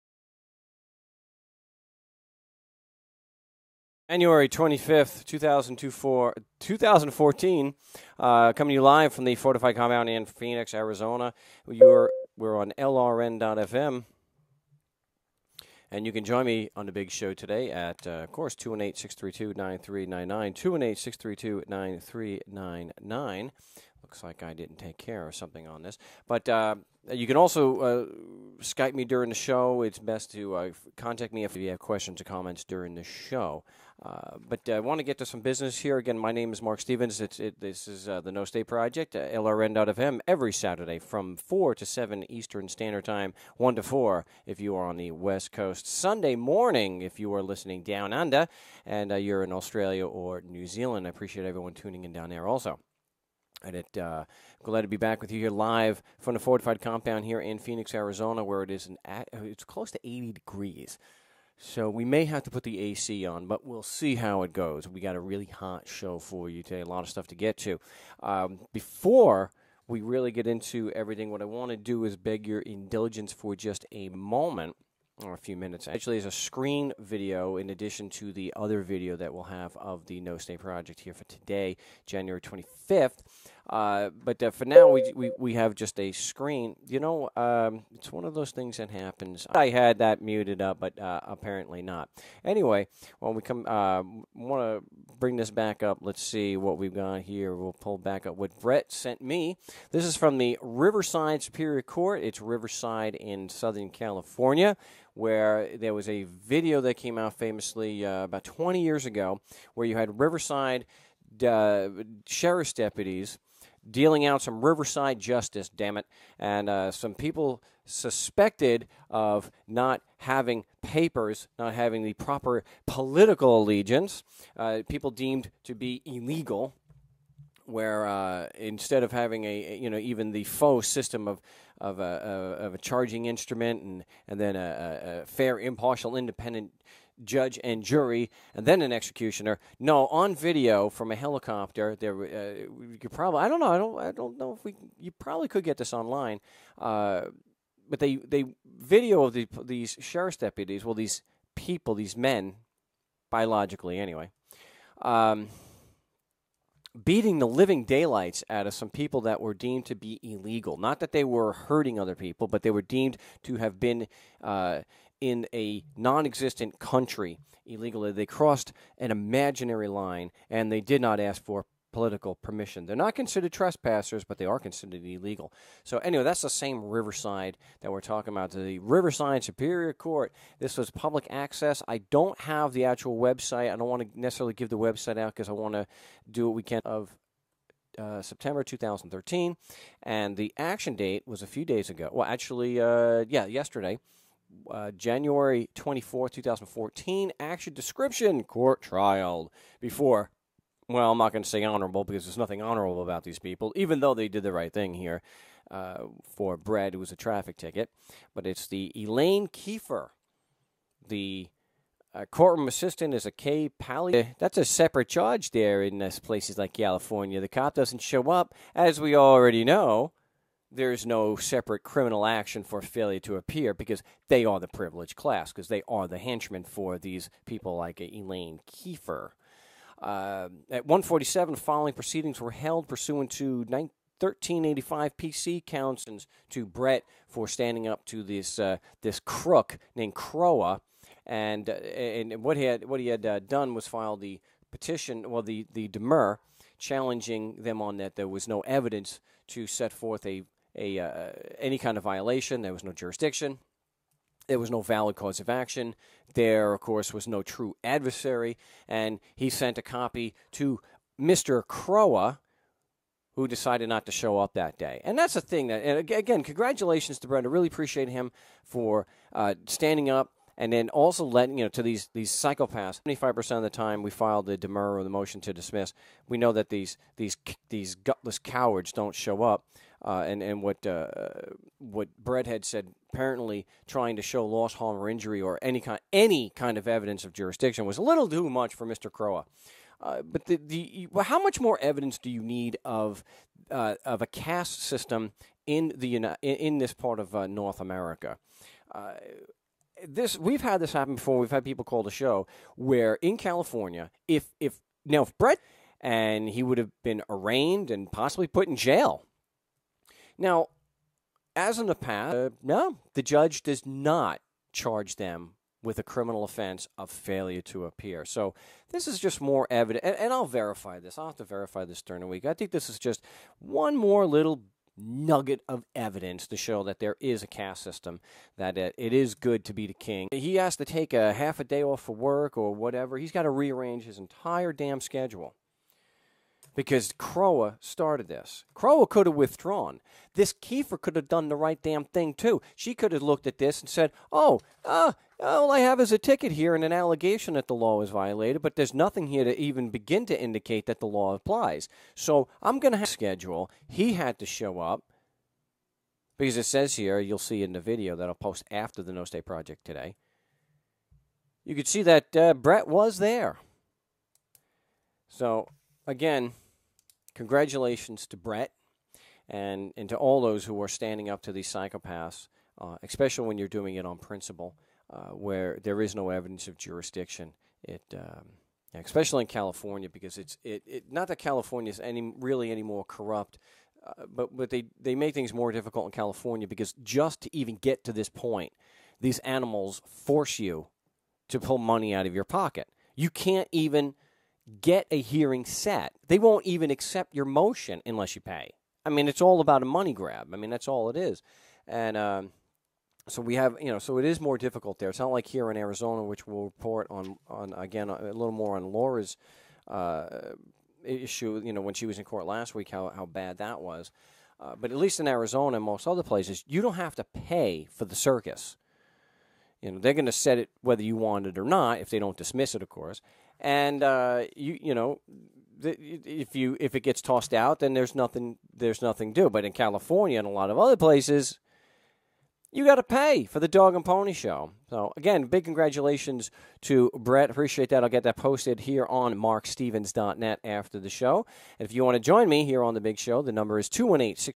Ticket Kicked in California - Congrats Brett
Congrats to Brett for standing up to the traffic predators and getting a ticket kicked out.  We spoke about this on the show for 25 January 2014.   Another example showing how effective it is to just challenge the arguments these parasites use to cover their crimes.  If there was evidence proving the "laws" of the "state" applied, then they would put it on the table.  Instead we have another parasite failing to show up to a hearing he caused to be set up.

Keep in mind this exposes the double standard so prevalent with these criminals: the predator responsible is not charged with a crime for failure to appear.  Had Brett not shown up you can guarantee he would be charged.  Not the hired guns of the "state" though.

Congrats Brett and to critics: if you have evidence the constitution and laws of the "state" of California apply to Brett or anyone else just because they are physically in California, then call into the show with your facts.